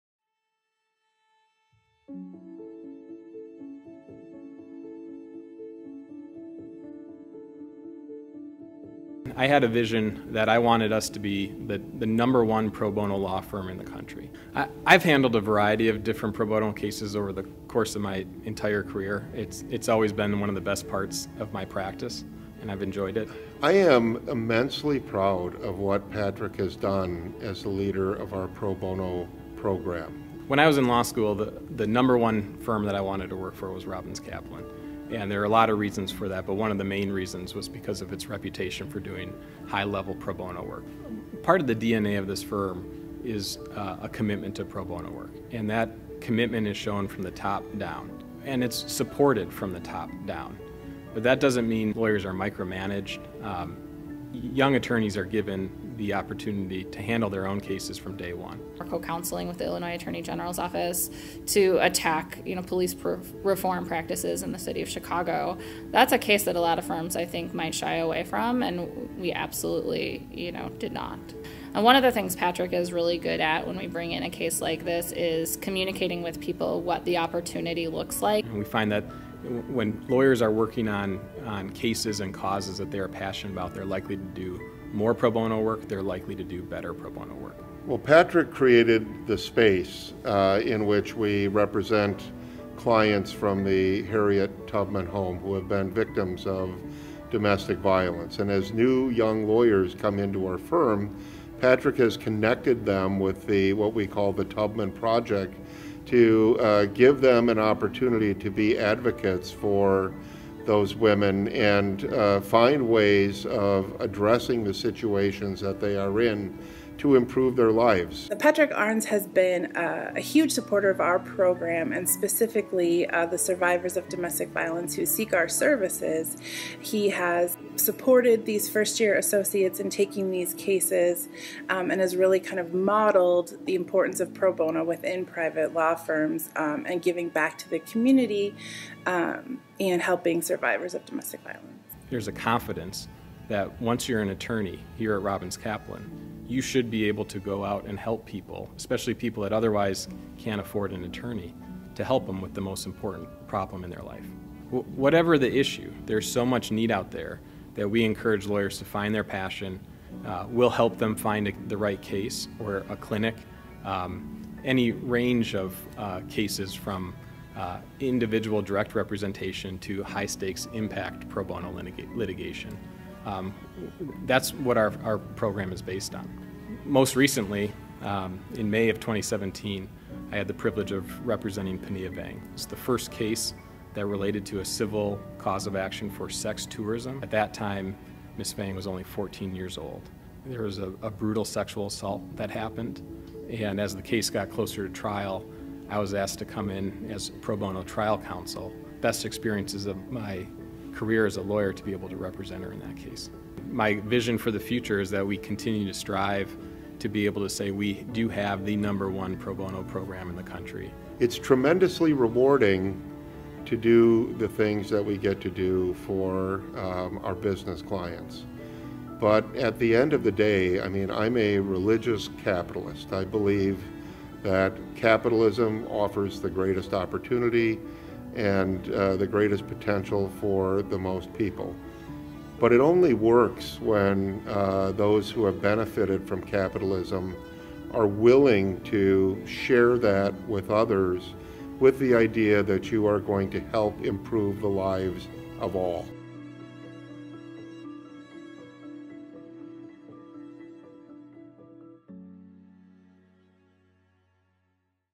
2018 - Patrick Arenz - Pro Bono Publico Award Recipient
Serving as a trial lawyer is a calling. A vocation that demands passion and commitment. I feel privileged to be a trial lawyer, and to have tried all sorts of cases in my career, including multi-million dollar verdicts against powerful companies. But among my many trials, I am most proud of several pro bono cases. As a first year lawyer, a young Ethiopian woman who faced persecution for her political views on women’s rights and free speech was granted asylum. A young man with the cards stacked against him needed vigorous representation against felony charges. And several years ago, a verdict in an international custody dispute under the Hague Convention allowed a young girl to continue living in the United States with her mother. The clients in each case all had the same question: can you help me with the most important problem in my life? I was honored then that they each entrusted me with their problem. And I am honored today to receive the ABA Pro Bono Publico award as recognition for this pro bono work.

The background for my pro bono work started 80 years ago. In 1938, two lawyers started a firm of their own because antisemitism denied them employment at established firms. From that day on, Robins Kaplan LLP has worked tirelessly to ensure equal access to justice for all. That mission motivated me to join the firm out of law school in 2006. Through my experience as Chair of the firm’s Pro Bono Committee, I have witnessed that mission in action. My colleagues have rewritten the odds for so many people who had no one else to turn to. I have seen some of our most significant wins, and shared in some of our toughest losses. Above all, my colleagues, from junior associates to senior partners, have answered the call—time and again—when someone needed a trial lawyer but could not afford one. I am so proud to be part of this firm.

My work for Panyia Vang remains the highlight of my career. Panyia brought her case in federal court against her abuser—a U.S. citizen who sexually assaulted her in Laos when she was 14 and he was 43 years old. She carried the trauma from that assault with her for years. Panyia pursued her case for years with unrelenting courage—confronting not only her abuser, but also the unsupportive community she lived in.

She testified with resolve for hours. Listening to her testimony, it was clear that emotion knows no language. Anyone who heard her testimony, even without translation from Hmong to English, could feel the powerful impact it had. The jury certainly did. Verdict means to speak the truth. And the jury’s $950,000 verdict spoke on behalf of the community that what the defendant did to Panyia was wrong and will not be tolerated.

This verdict sends a broader message too. “Sex tourism,” referred to as international travel for illicit sex, is an unconscionable crime. And it occurs more often than any of us can imagine. The U.S. State Department reports that sex tourism is a $1 billion a year industry. Panyia and I hope this verdict makes the next perpetrator think twice before getting on a plane to head overseas—and if not, that he knows he may be held accountable by a jury in federal court.

There is not enough space to express my gratitude for those who helped me develop my career as a trial lawyer. In retrospect, however, any achievements I have obtained all stem from my Mom and Dad—the opportunities they provided and the values they instilled in me, including the importance of treating everyone with dignity and respect. Today, no one sacrifices more for me and our children than my wife, Christine, who celebrates the good days and shoulders the bad ones, but no matter what, is always by my side. I am also grateful for the mentorship that Marty Lueck and Ron Schutz have provided throughout my career. They trained me as a trial lawyer, and I have used the skills they helped me develop in every one of my pro bono cases.

Few professions offer the ability to change people’s lives forever. Being a trial lawyer is one that does. And pro bono work offers the ability to do that for those who have no other hope—the poor, the disadvantaged, those who live in the shadows of life. My past pro bono work (and this award) will always serve as a reminder to me, as I hope it does for others and especially my children when they grow older, that when someone in need asks if we can help them, that the answer is an easy one.